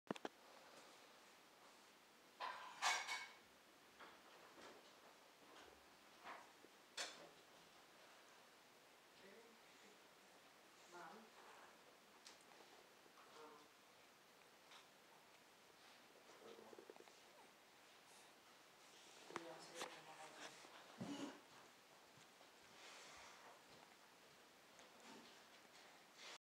Baby Layla Can Feed Herself!
Baby Layla is 4 months old and can hold her own bottle and feed herself